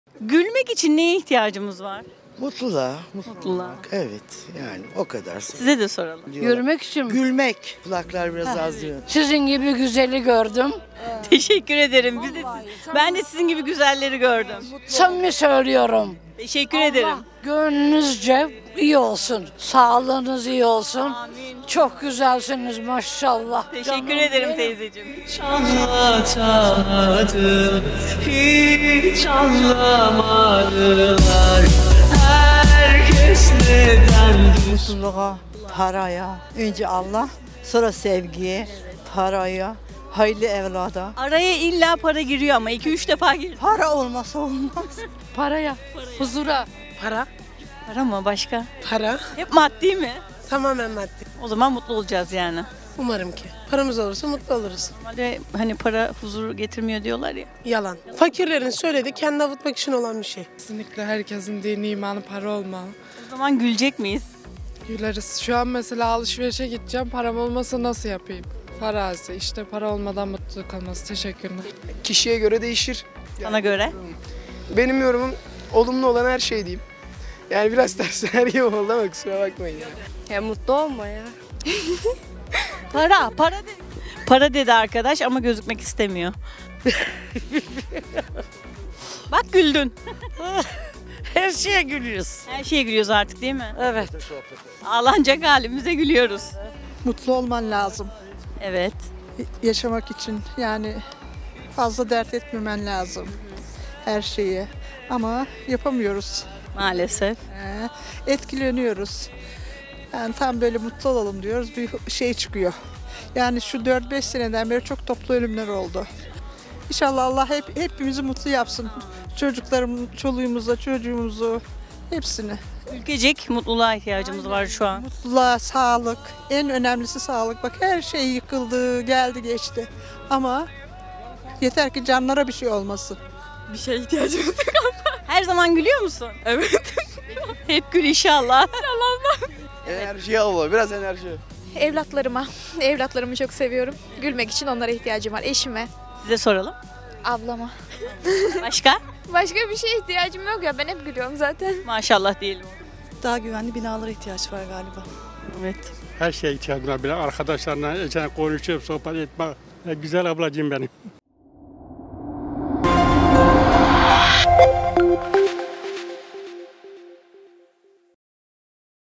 Gülmek için neye ihtiyacımız var ?
Balıkesir'li vatandaşlarımıza sorduk.
İlginç cevaplar aldık
İYİ SEYİRLER...